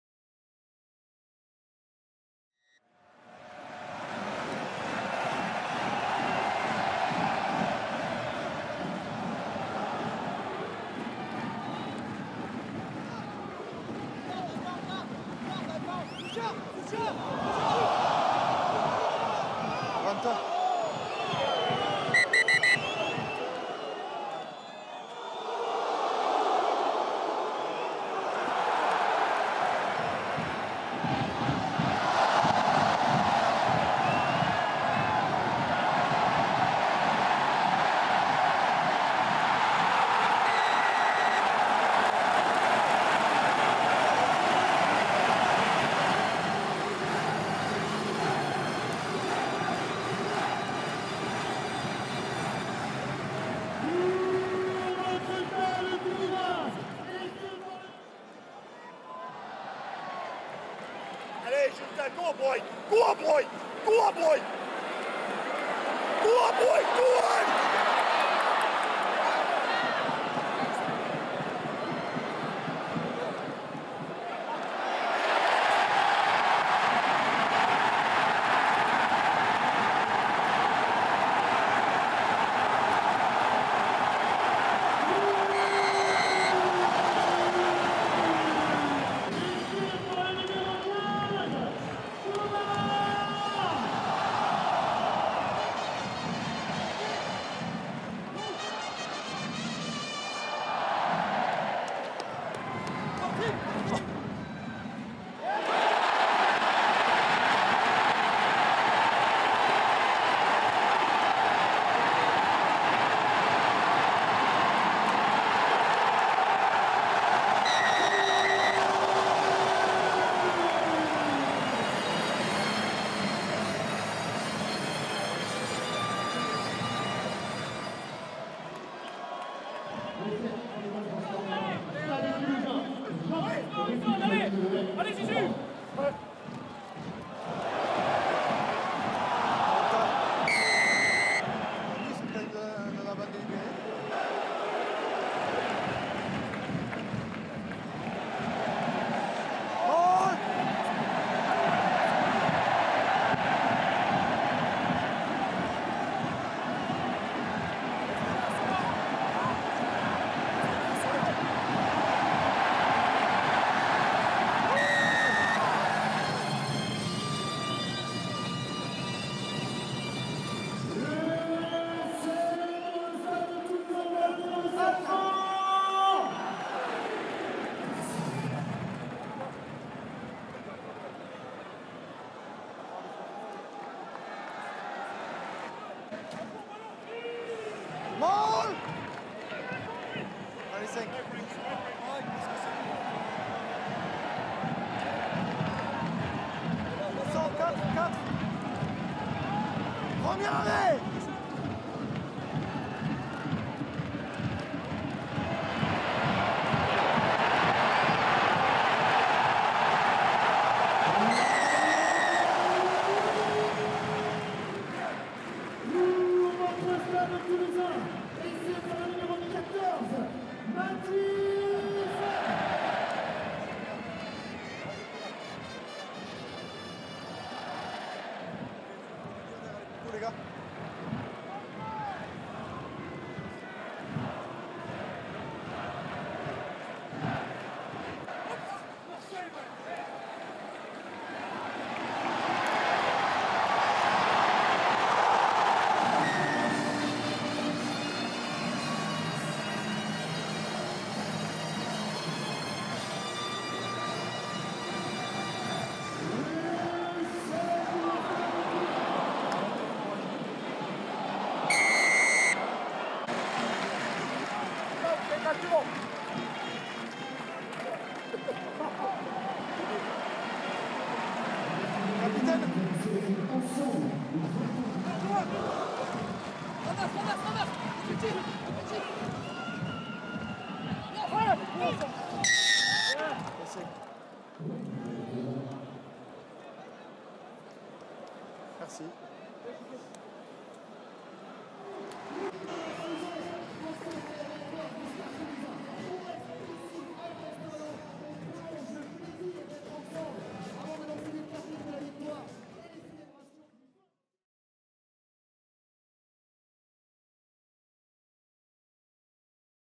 ST - RCT : le résumé du match
⚡️ 1ère victoire à domicile

😍 Retrouvez les 6 essais de la rencontre dans votre résumé de match,